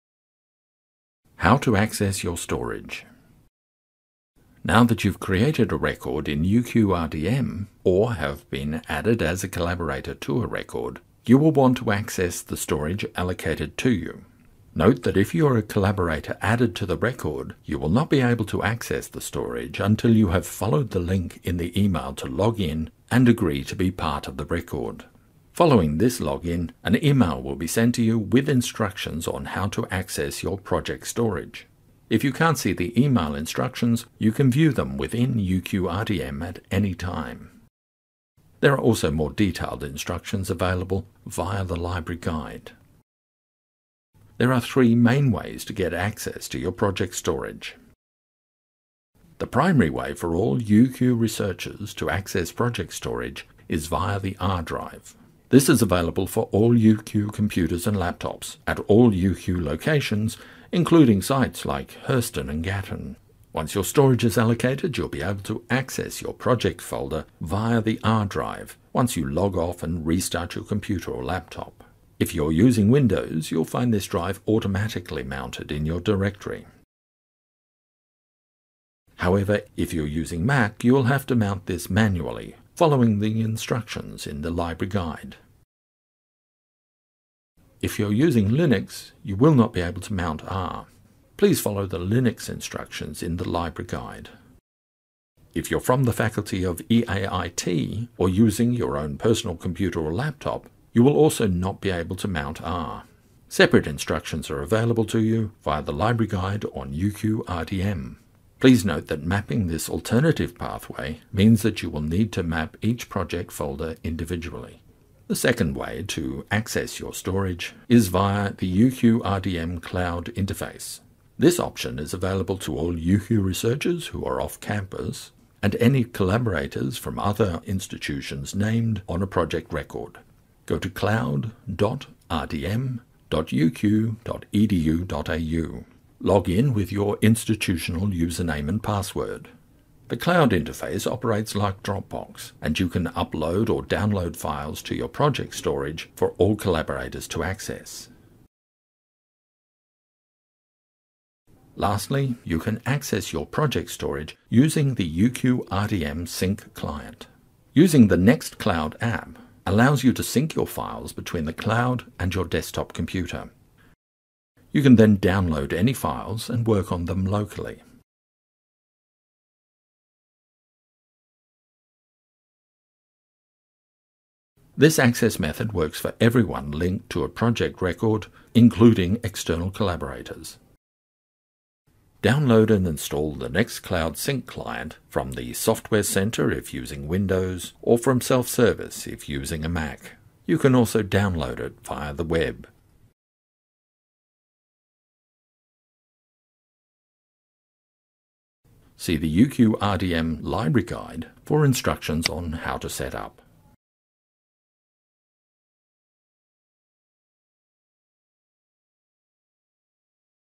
Access your storage for the UQ Research Data Manager
How to access your project’s storage now that you have created a record in the UQ Research Data Manager.